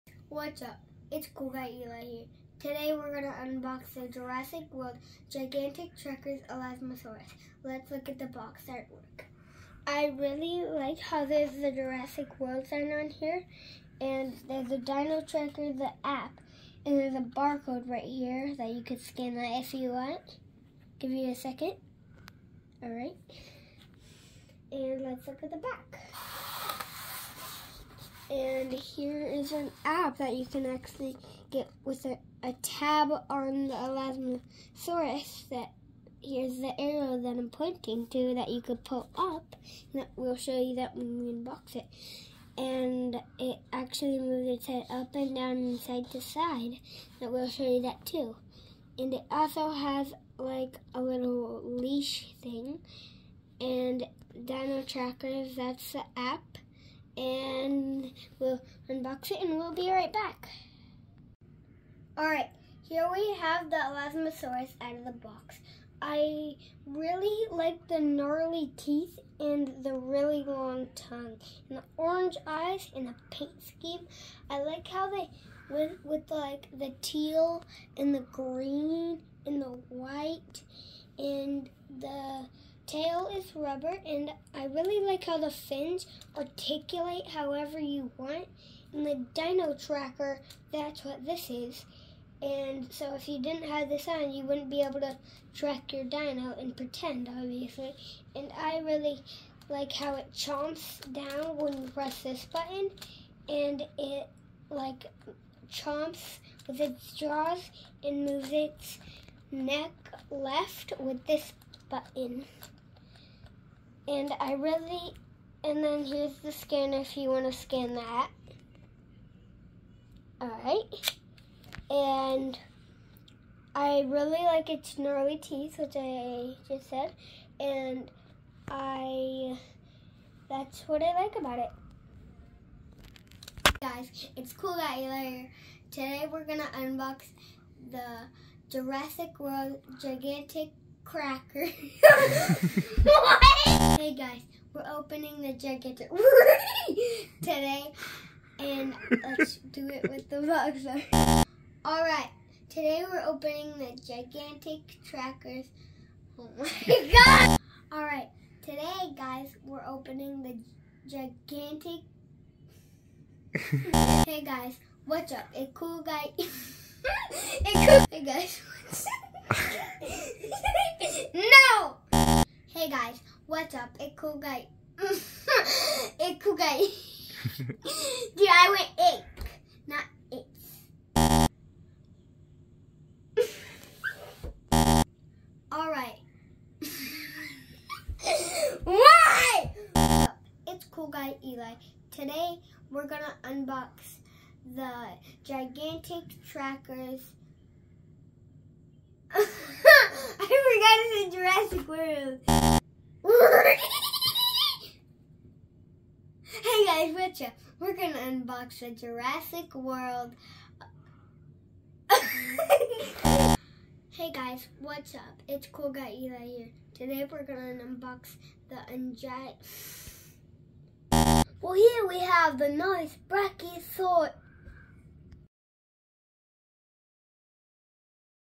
Jurassic World Gigantic Dino Trackers Elasmosaurus Review.
In this video Cool Guy Eli offers his review of The Jurassic World Gigantic Dino Trackers Elasmosaurus toy... Bloopers Included!